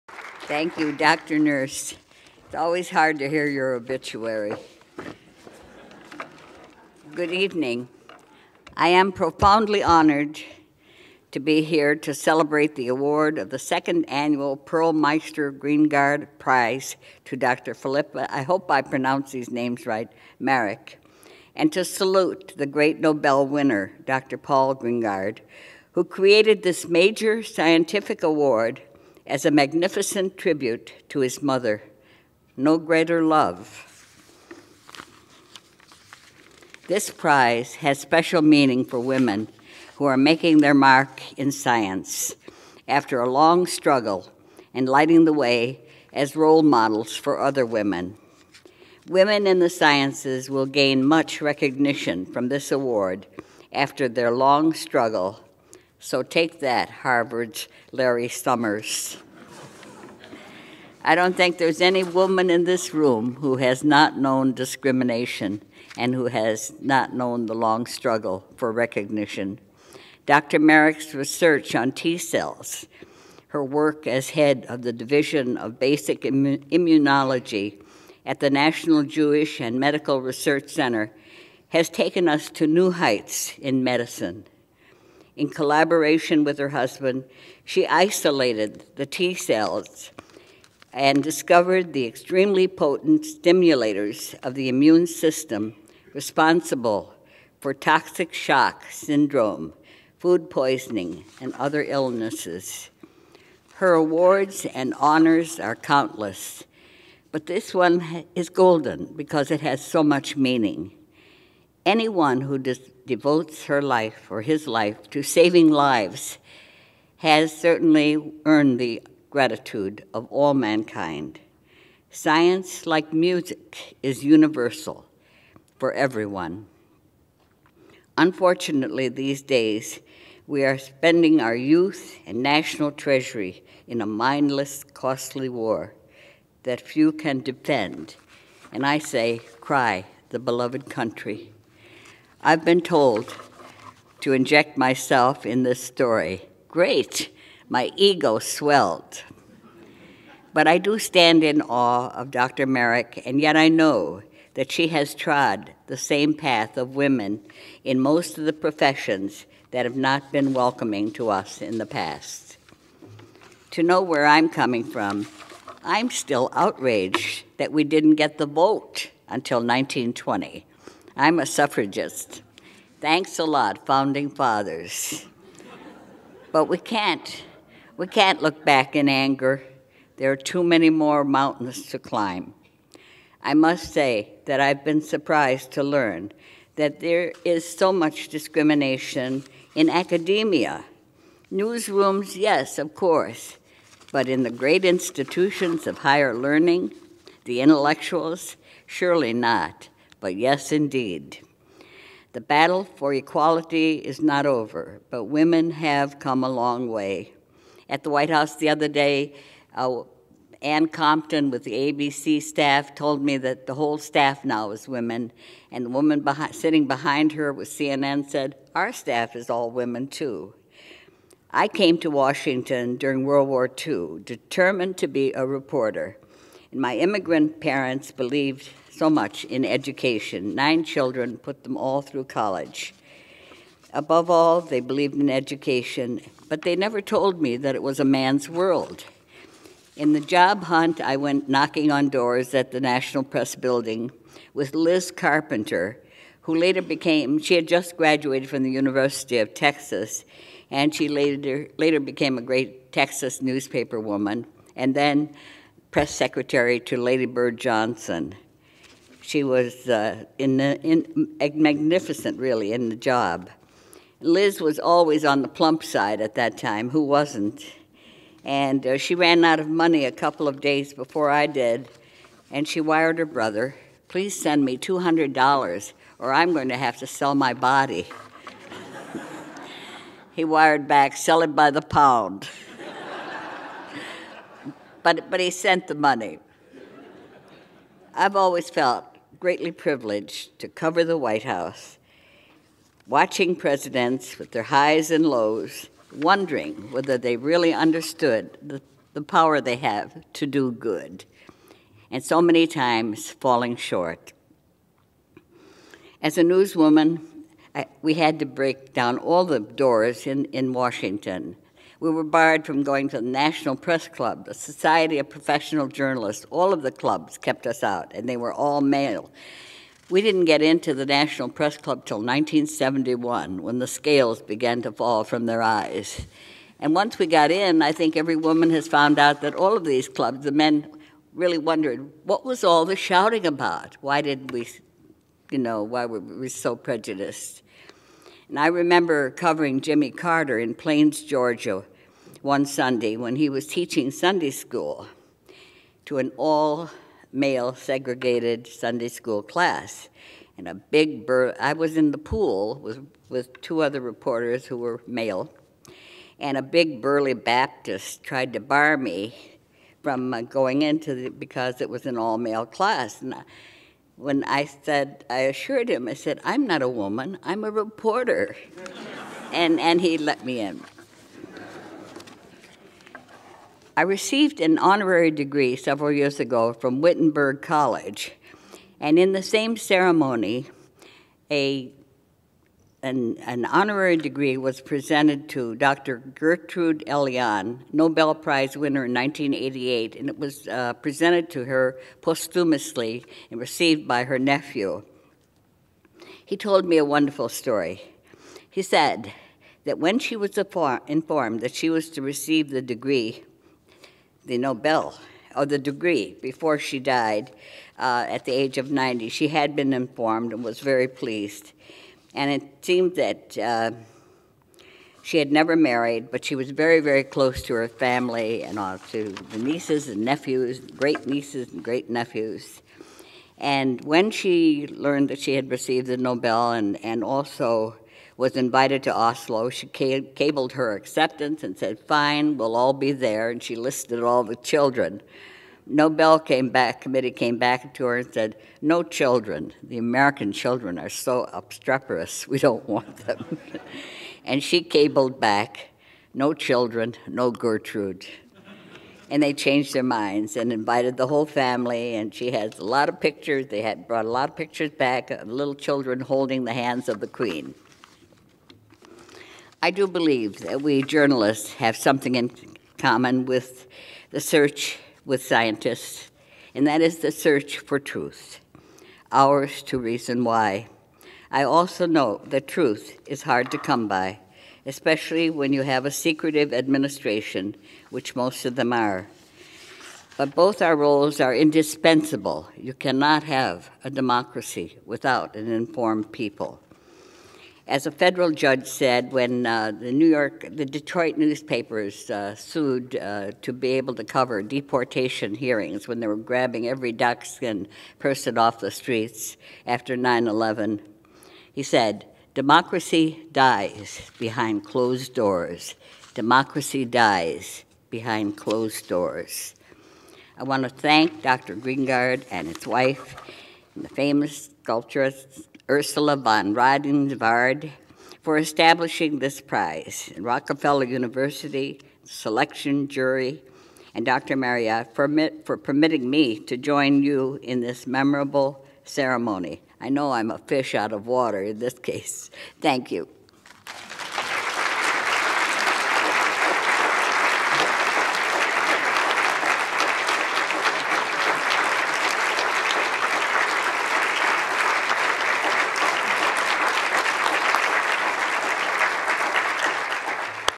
2005 Pearl Meister Greengard Prize - (Part 2)
The 2005 Pearl Meister Greengard Prize was awarded to Dr. Philippa Marrack for her numerous discoveries in the field of immunology. The guest presenter was Helen Thomas. Dr. Titia de Lange introduced the recipient.

The Pearl Meister Greengard Prize, awarded annually by The Rockefeller University, was established by Dr. Paul Greengard, the University's Vincent Astor Professor, and his wife, the sculptor Ursula von Rydingsvard. Named in memory of Dr. Greengard's mother, who died giving birth to him, the Pearl Meister Greengard Prize provides recognition of the accomplishments of women in science.